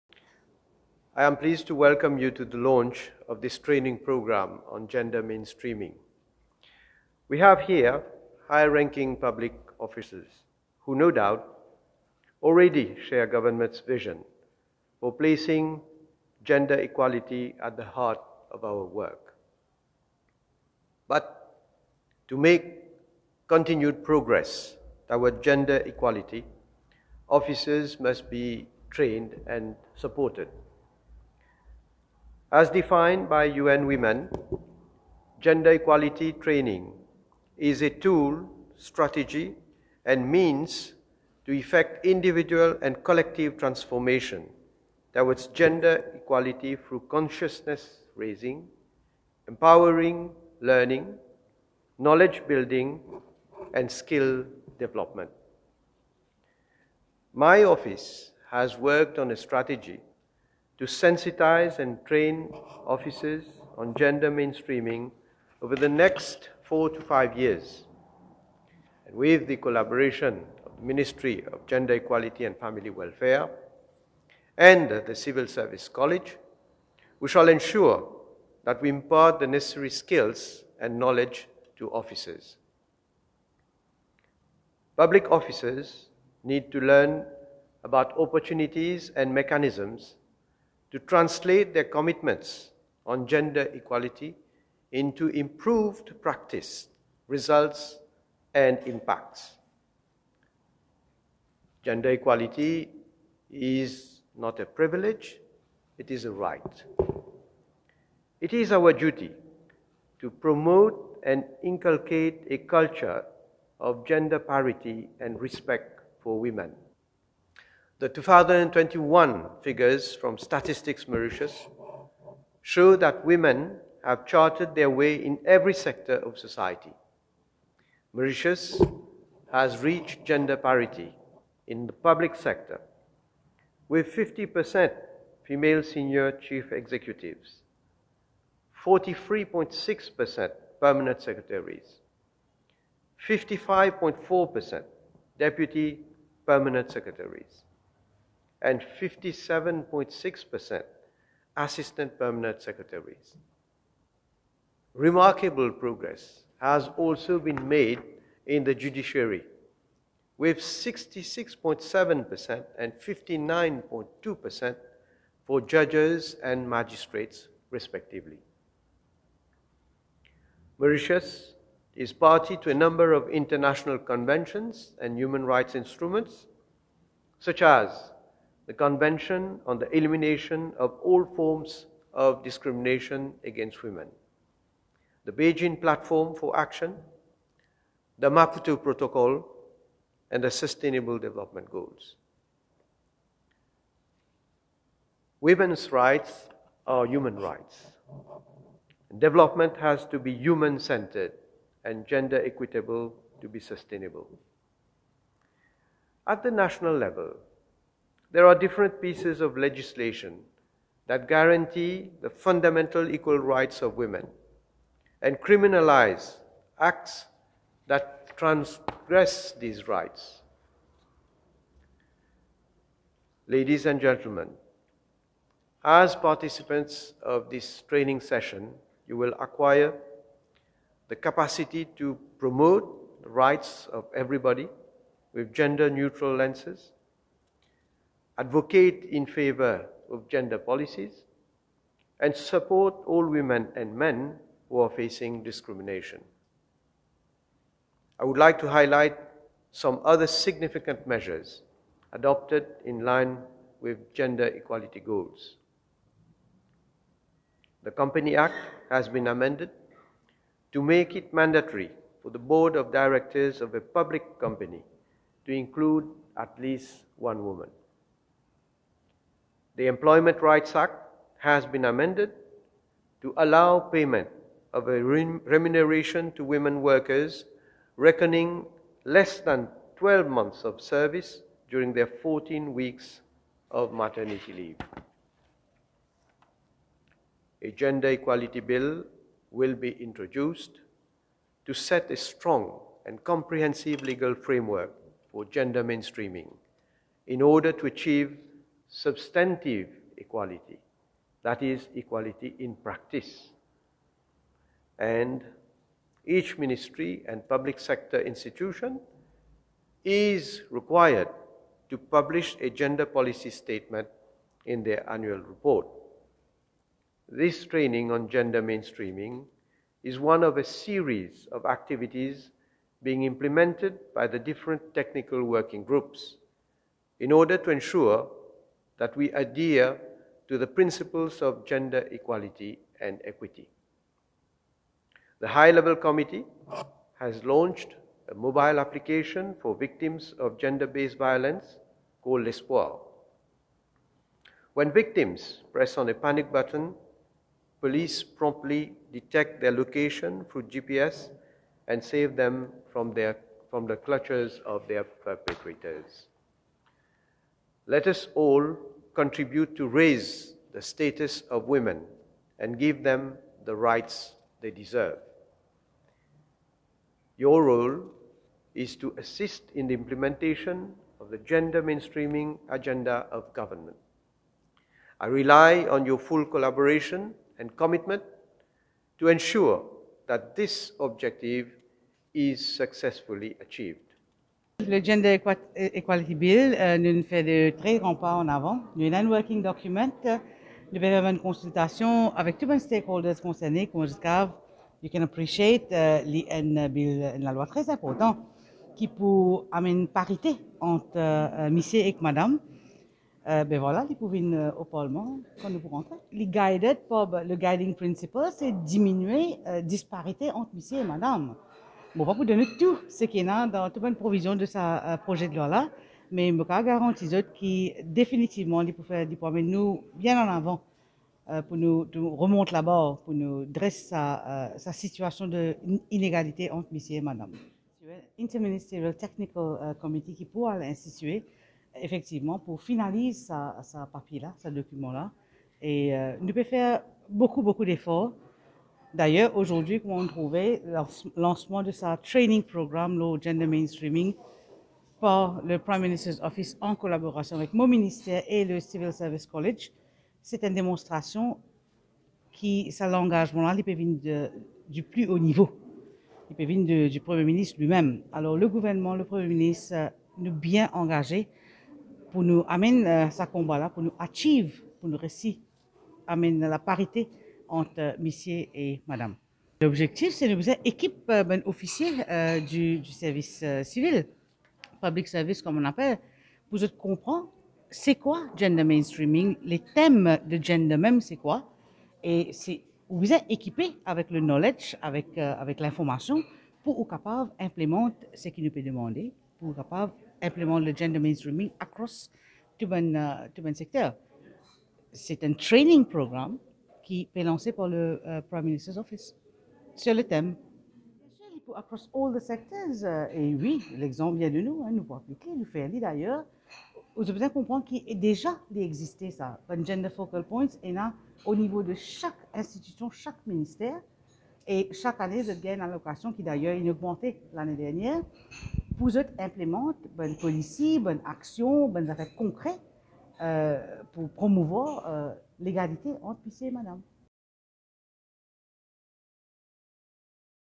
The Prime Minister launches training programme on Gender mainstreaming
The Prime Minister, Mr Pravind Kumar Jugnauth, launched, today, a training programme for Officers of the Prime Minister’s Office (PMO) on Gender mainstreaming at the Sir Harilal Vaghjee Memorial Hall in Port-Louis. The Minister of Gender Equality and Family Welfare, Mrs Kalpana Devi Koonjoo-Shah and other personalities were also present at the launching ceremony.

This training session is being organised by the Citizen Support Unit, under the aegis of the PMO, in collaboration with the Ministry of Gender Equality and Family Welfare and the Civil Service College, Mauritius (CSCM).

In his keynote address, Prime Minister Jugnauth highlighted that Government’s vision is to place gender equality at the heart of its work, but in order to make continued progress towards gender equality, Officers must be trained and supported. The PMO, he stated, has worked on a strategy to sensitise and train Officers on gender mainstreaming over the next four to five years and with the collaboration of the Ministry of Gender Equality and Family Welfare and the CSCM, Government will ensure that the necessary skills and knowledge are imparted to Officers.

Participants of this training session, he underscored, will acquire the capacity to promote the rights of every citizen with gender neutral lenses, advocate in favour of gender policies, and support all women and men who are facing discrimination. Today’s platform, he pointed out, is also an opportunity to share with participants some of the concrete actions taken to advance Government’s gender equity agenda.

Furthermore, the Prime Minister underlined that this training is one of a series of activities being implemented by the different technical working groups in order to ensure that public Officers adhere to the principles of gender equality and equity. He observed that the role of participants is to assist in the implementation of Government’s gender mainstreaming agenda.  Government relies on their collaboration and commitment so as to ensure that this objective is successfully achieved, he emphasised.

In addition, Prime Minister Jugnauth dwelt on statistics which show that women have chartered their way in every sector of society, where remarkable progress has been made in the public sector and the judiciary.  He likewise recalled that some significant measures were adopted in line with gender equality goals. He further reassured that one of Government’s objective is also to eliminate gender-based violence in Mauritius.

For her part, Minister Koonjoo-Shah remarked that this training will set the scene to provide better insights to participants on how to integrate gender perspective in all their interventions at the workplace. She expressed her wish that this example is emulated by other Ministries in implementing value-added and gender sensitive actions through the funds, to the tune of Rs 200,000, which are allocated to each of them.

Mrs Koonjoo-Shah indicated that today’s training will not only bring the requisite knowledge but also a better understanding and ease of implementing meaningful changes for a more inclusive society. She also stated that her Ministry is in the process of finalising the National Gender Policy for the period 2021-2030 and an inter-ministerial gender committee will be set up to provide sustained guidance to the gender focal points.